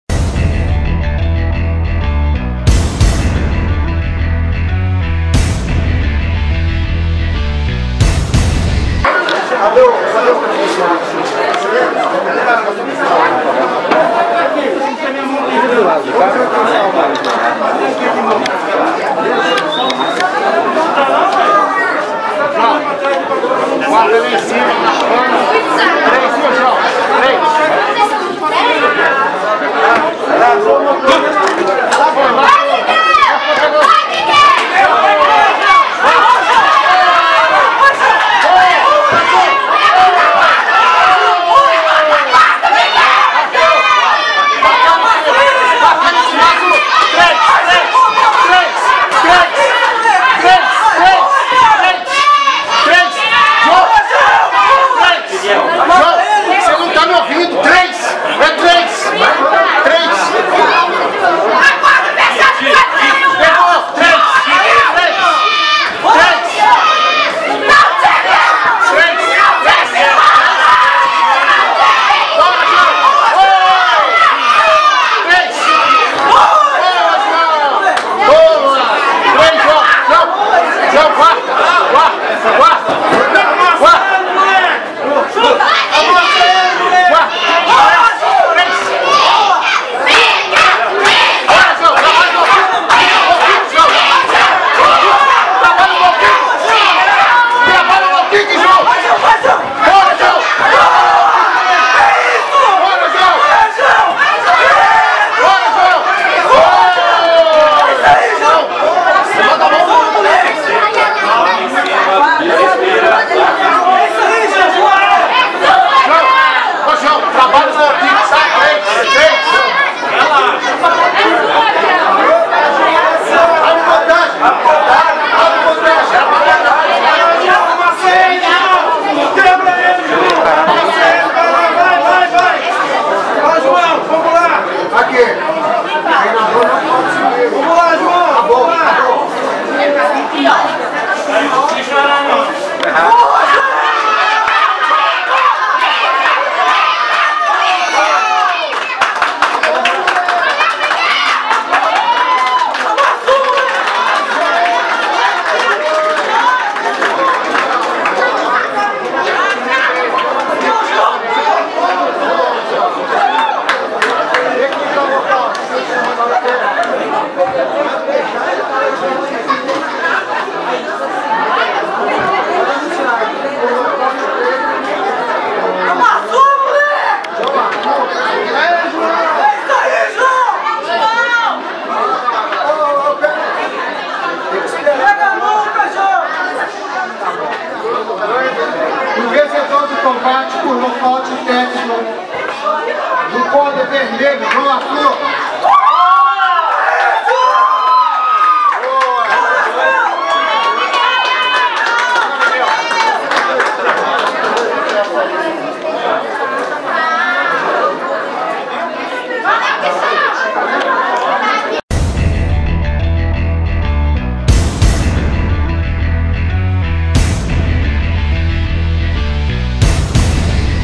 Miguel vs João - Champions Fight Kids 2
João Arthur (Águia Muay Thai) venceu Miguel (Shadaloo Muay Thai Gym) por nocaute técnico.